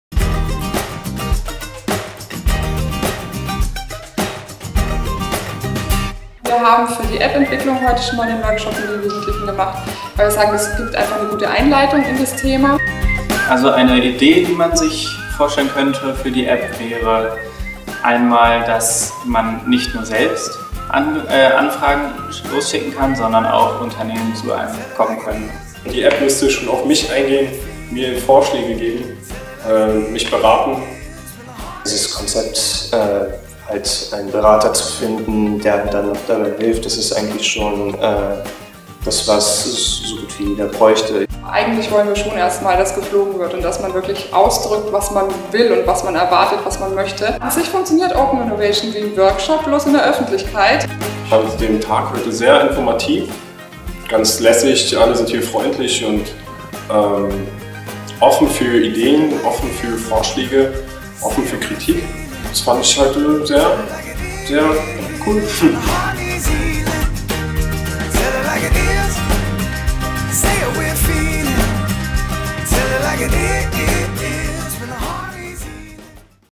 Schülerworkshop in Berlin: So spannend ist App-Design
Was Jugendliche bei der Suche nach einem Ausbildungsplatz wirklich brauchen?

Das fragen wir sie am liebsten selbst! Vorschläge können auf der Ideenwerkstatt der Bundesagentur für Arbeit geäußert werden:

Die aktivsten Teilnehmer haben außerdem die Chance tolle Preise zu gewinnen: 2-tägige Schnupperpraktika in bekannten Unternehmen, Lautsprecherboxen fürs Handy oder eine Virtual Reality Brille fürs Smartphone!

Das Video zeigt den Workshop zum „App-Design“ am 07.Oktober 2015 mit einer Schülergruppe in Berlin. Hier wurden gemeinsam mit den Teilnehmern erste Ideen gesammelt, wie die neue App der Bundesagentur für Arbeit aussehen soll. Der Workshop bildete den Auftakt zum Projekt Auszubildenden-App in dem diese Ideen mit einem breiten Publikum weiterentwickelt und neue Ideen generiert werden sollen.